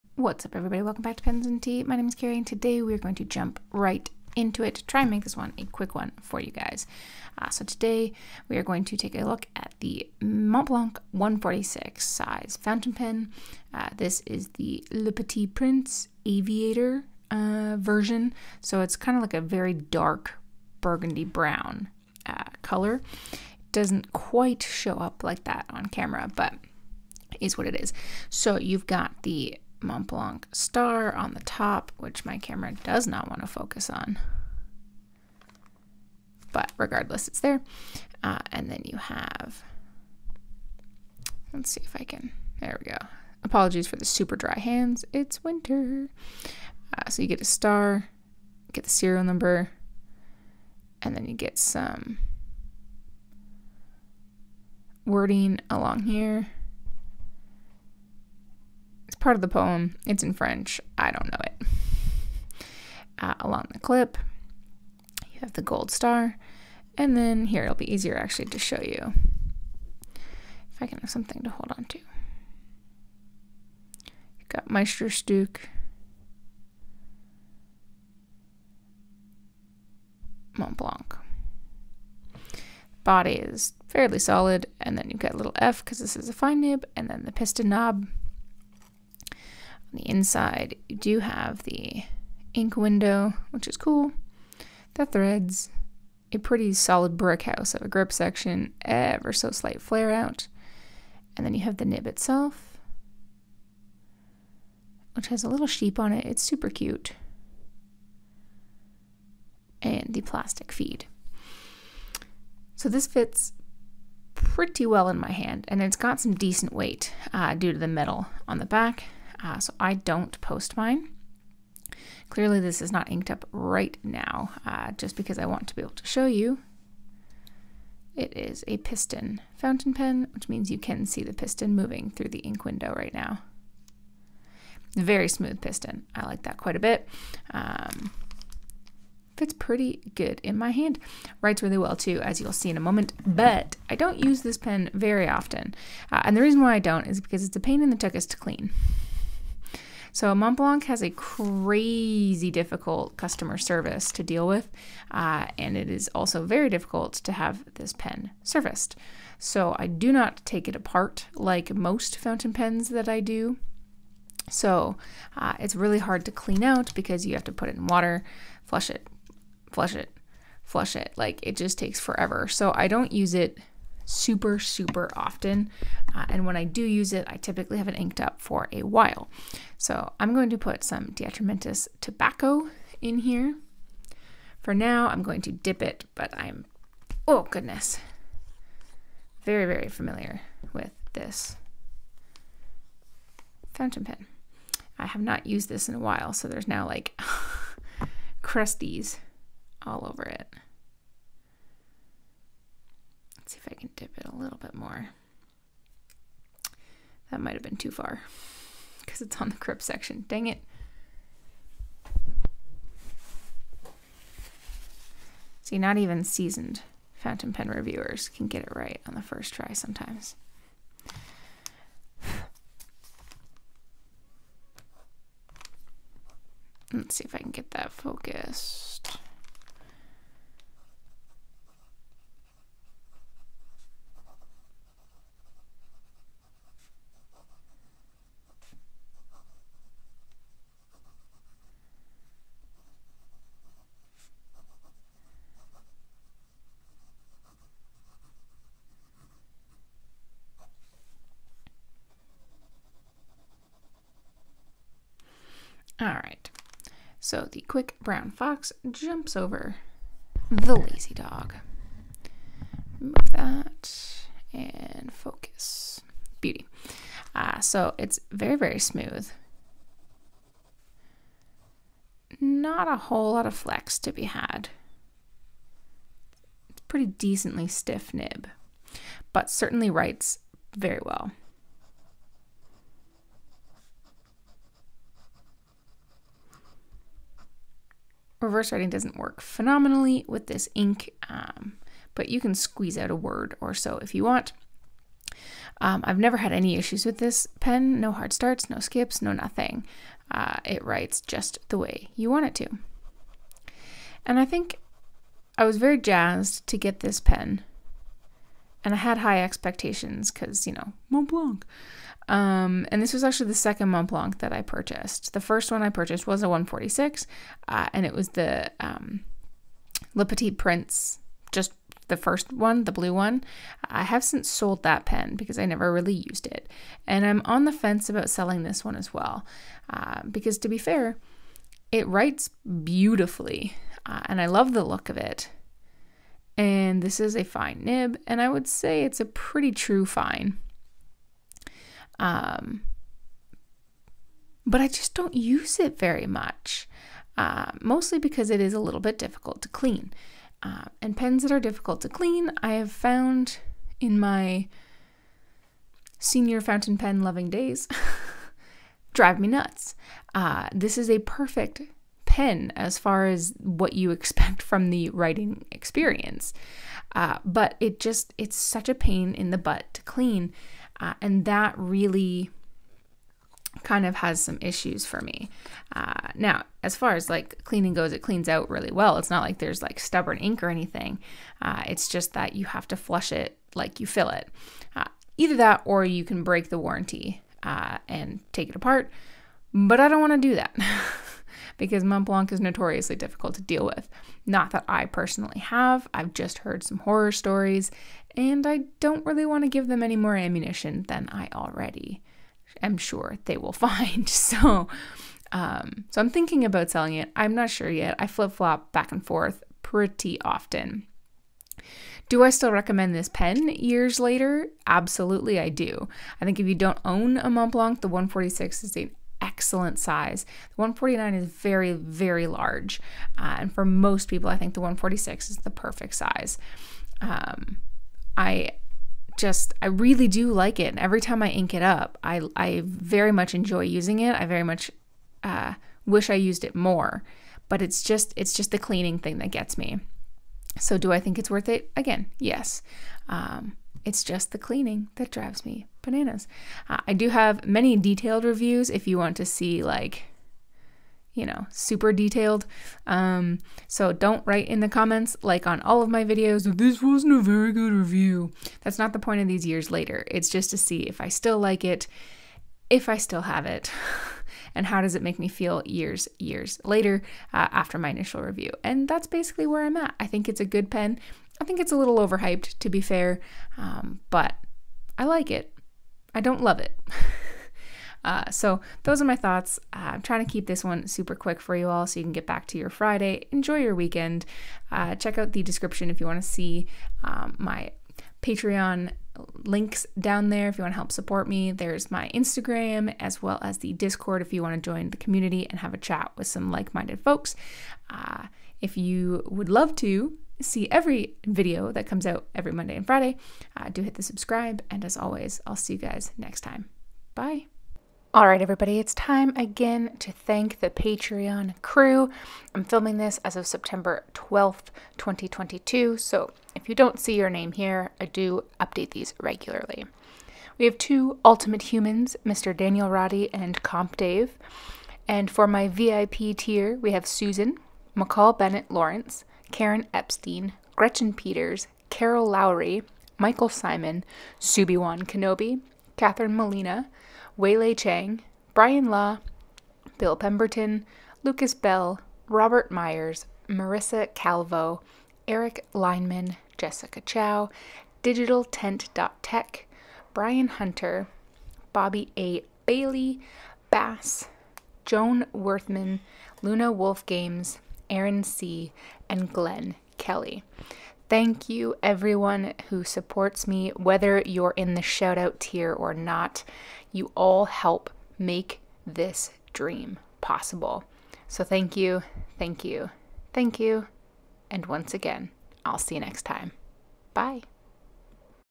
Years Later: Montblanc 146 re review
I am so happy that you all are loving this Years Later review series. What do you want to see next?